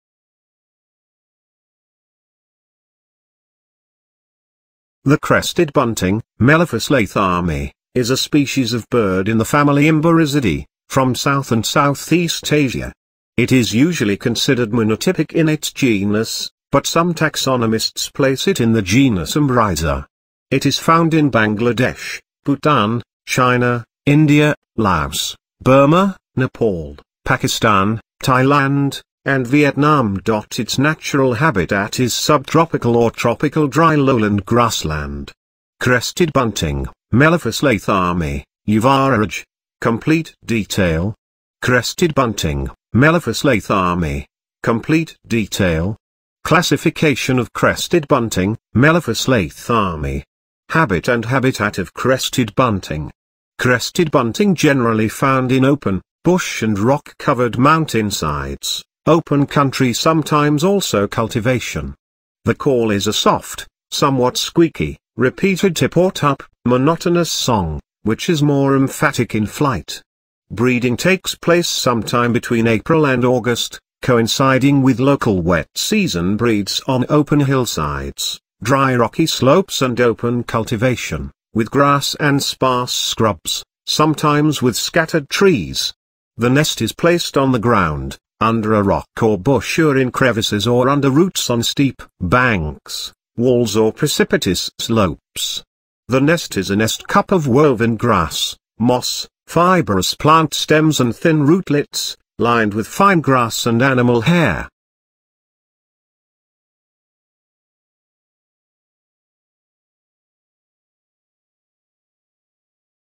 Interesting facts Bunting crested by weird square
The crested bunting (Melophus lathami) is a species of bird in the family Emberizidae, from South and Southeast Asia. It is usually considered monotypic in its genus, but some taxonomists place it in the genus Emberiza.
It is found in Bangladesh, Bhutan, China, India, Laos, Burma, Nepal, Pakistan, Thailand, and Vietnam.Its natural habitat is subtropical or tropical dry lowland grassland.
Crested bunting (Melophus lathami) Yuvaraaj – complete detail. Crested bunting (Melophus lathami) complete detail. Classification of Crested bunting (Melophus lathami). Habit and habitat of Crested bunting. Crested bunting generally found in Open, bush and rock-covered mountainsides, open country sometimes also cultivation. The call is a soft, somewhat squeaky, repeated tip or tup, monotonous song, which is more emphatic in flight. Breeding takes place sometime between April and August, coinciding with local wet season
Breeds on open hillsides, dry rocky slopes and open cultivation, with grass and sparse scrubs, sometimes with scattered trees. The nest is placed on the ground, under a rock or bush or in crevices or under roots on steep banks, walls or precipitous slopes. The nest is a nest cup of woven grass, moss, fibrous plant stems and thin rootlets, lined with fine grass and animal hair. Animal planet | Discovery | Netflix | Documentary | Animal World